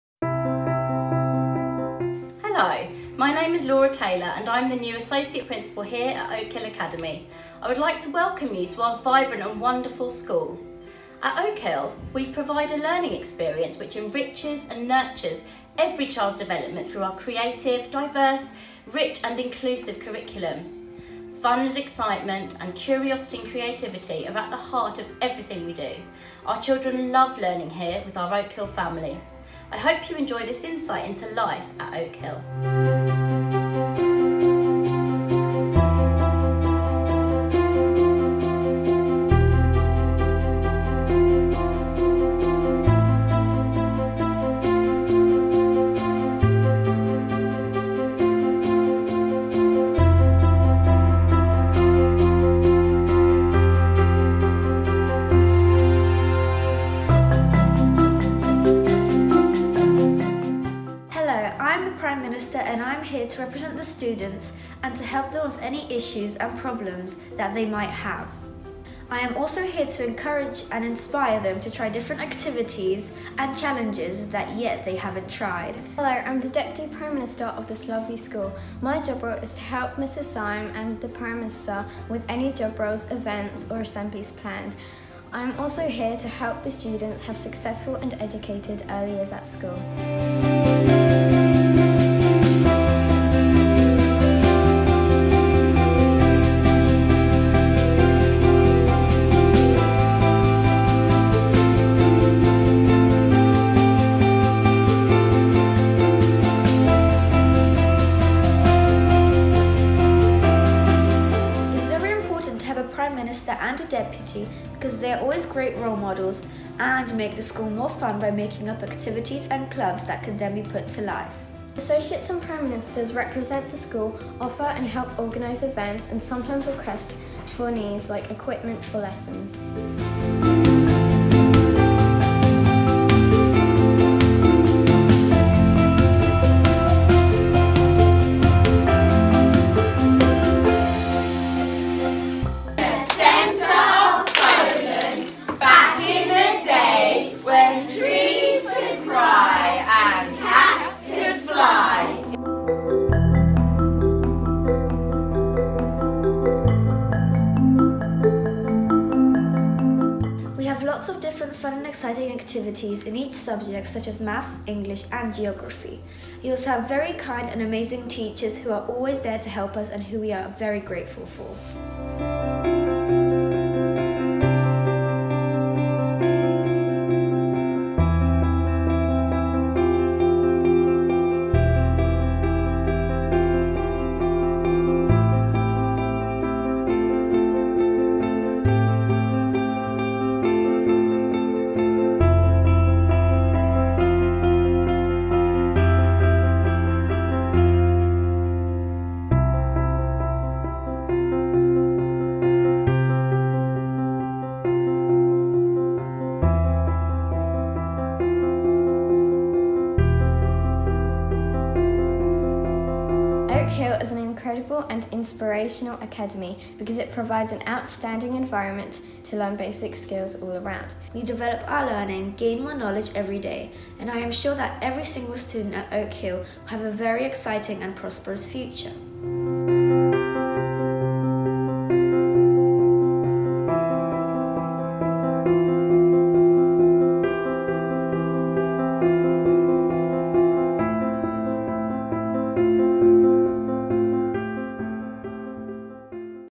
Life at Oak Hill Academy
Enjoy this video as an insight into life at Oak Hill Academy!

At Oak Hill, At Oak Hill, we strive for the highest academic achievement and enable children to achieve excellent results whilst with us.

We nurture our children and our staff; everyone is valued and supported as part of our Oak Hill family and we are all each other’s heroes!